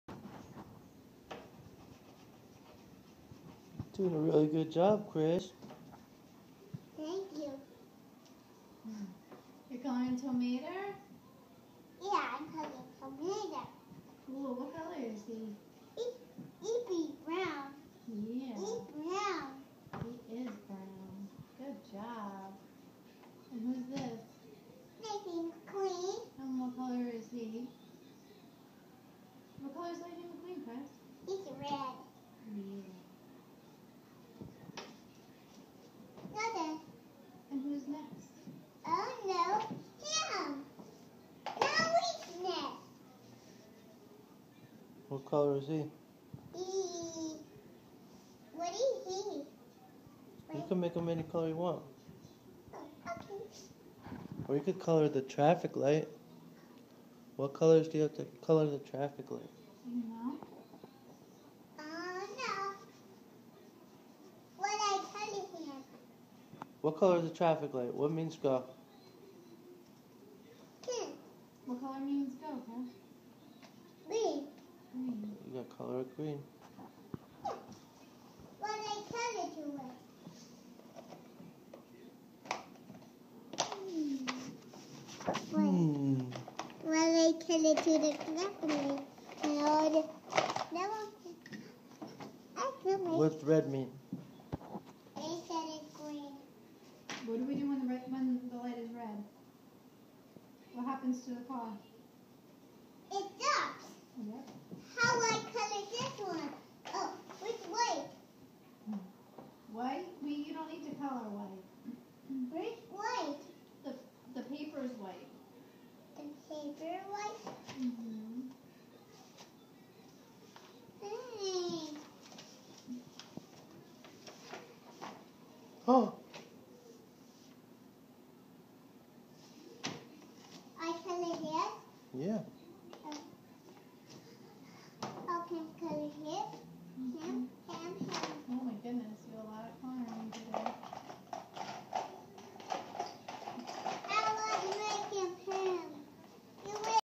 Chris coloring
1-18-14
Chris coloring a card he got for his birthday